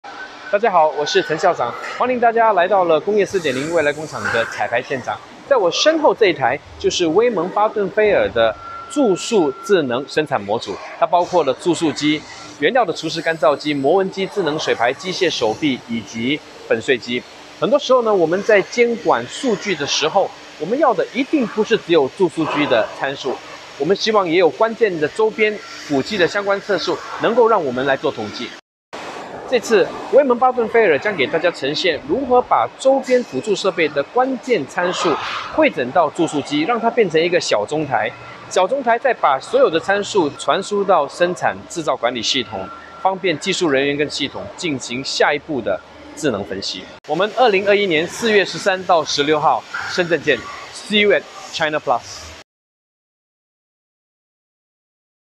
【Industry 4.0 Factory of the Future】All Key Parameters Of Injection Molding Machine And Peripheries
【Industry 4.0 Factory of the Future | 工业4.0未来工厂】
All The Key Parameters Of Injection Molding Machine And Peripheries Are Here! | 不止注塑机参数，这里汇整了所有关键数据！

Most of the time, when we monitor the production data, we require more than just the process parameters of the injection molding machine, but also the key parameters of peripheries for analysis.

In CHINAPLAS 2021, WittmannBattenfeld will demonstrate how Wittmann 4.0 platform integrates the production information of both the injection molding machine and its peripheries on the machine console.

This premier concurrent event in CHINAPLAS 2021 is organized by Adsale and iPlast4.0, and jointly organized by EUROMAP, VDMA, and OPC Foundation.

大家在监管数据的时候，不止注塑机的参数，是否也希望有关键的周边辅机的相关参数来做统计？

CHINAPLAS 2021 国际橡塑展 同期活动之一 -工业4.0未来工厂钻石合作伙伴之一 威猛巴顿菲尔 Wittmann Battenfeld将为大家呈现透过模块化的威猛4.0功能，其质量监控信息可与智造监控中控台整合。注塑机能够访问周边设备的参数和运行状态，方便技术人员跟系统进行下一步的智能分析。

CHINAPLAS2021 | The World's Leading Plastics & Rubber Trade Fair